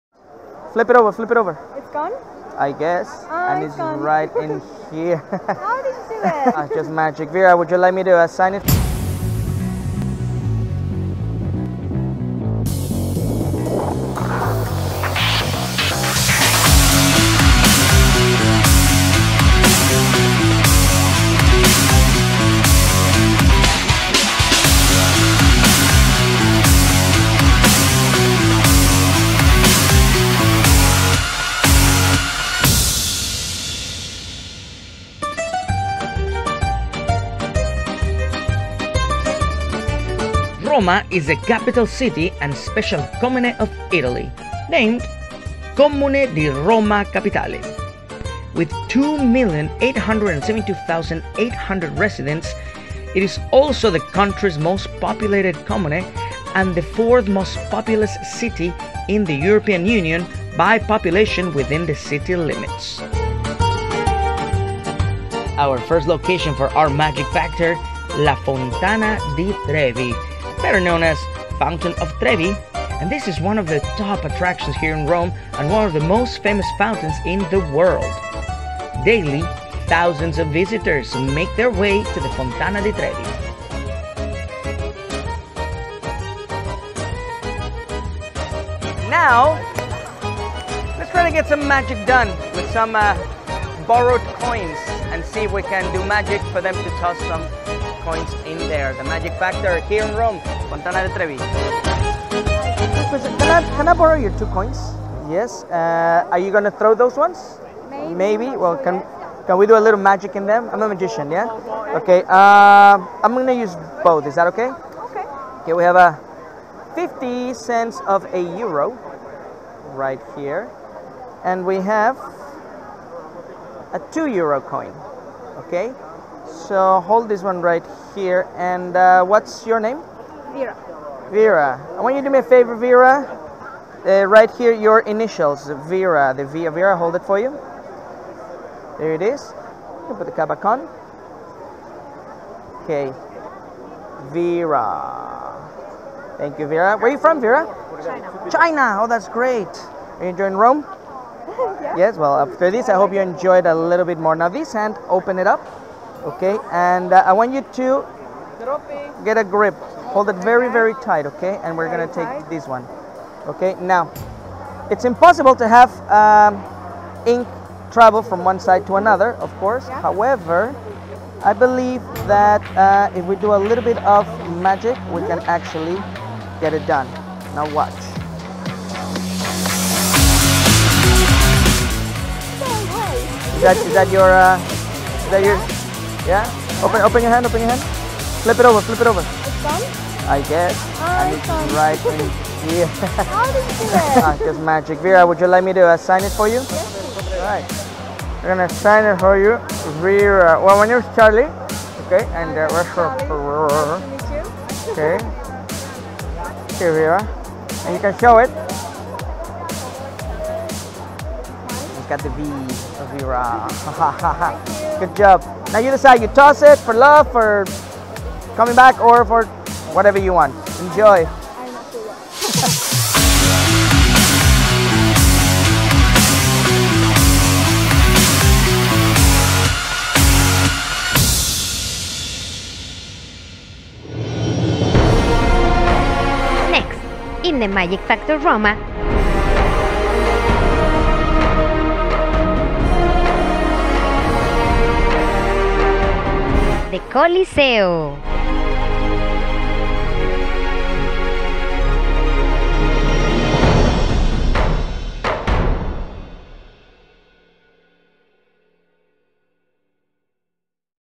Coin Magic at the Fontana di Trevi, Roma
This time Charly D'Carlo will do something unusual with 2 borrowed and signed coins, check it out!